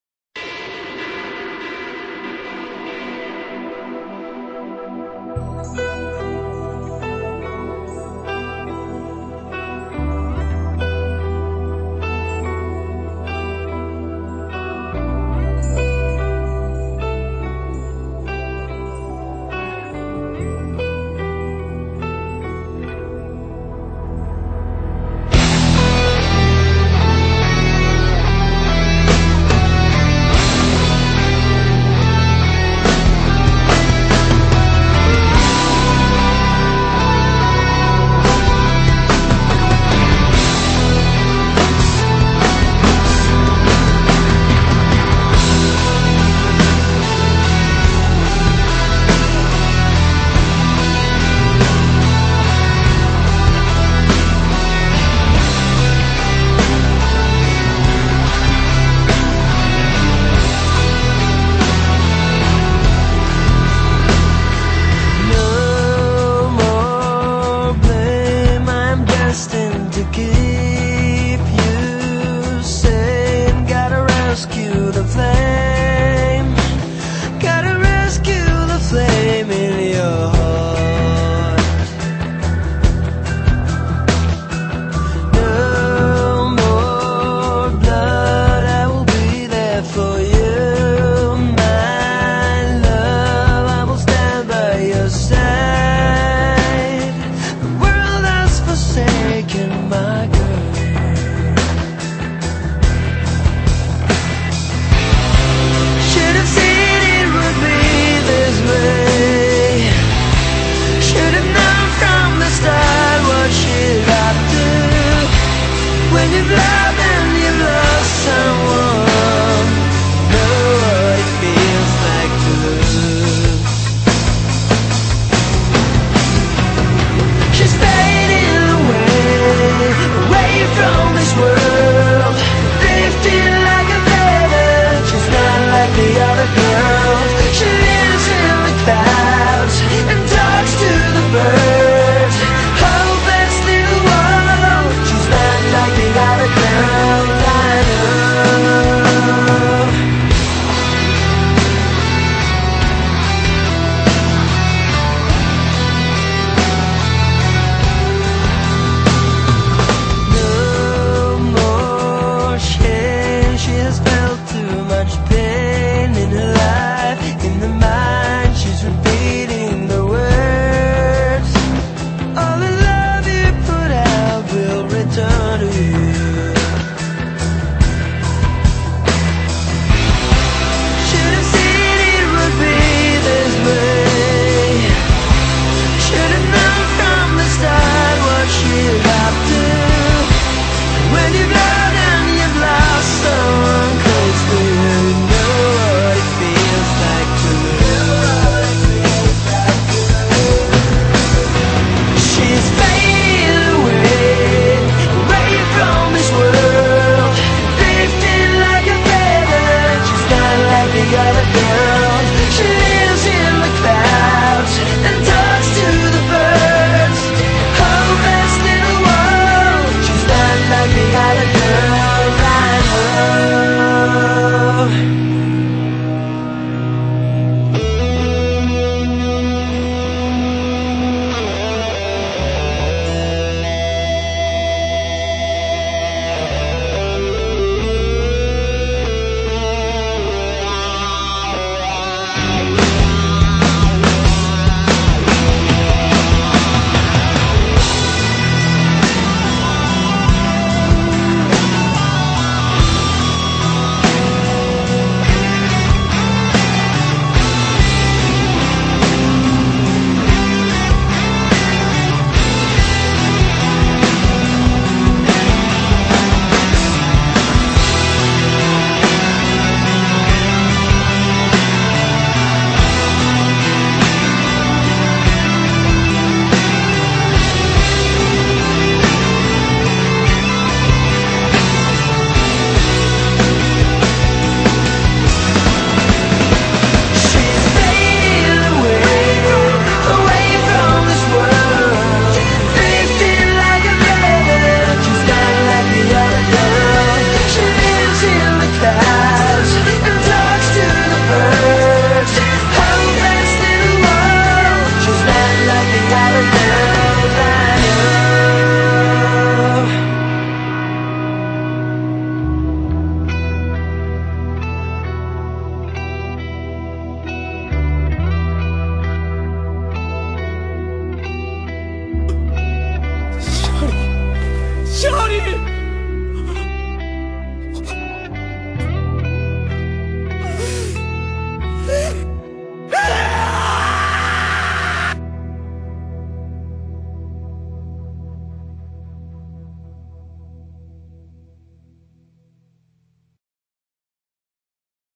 ~Shirley's fading away~
Shirley and Lelouch are my favorite characters and couple I made a video about them.Please without bad comments.I worked really hard and it took me so many hours to make this video.Hope you like it.
There is not anything special about the plot.
The black/white screens are memories and the color ones are the present.

~R.I.P. Shirley ~

~wish I owned~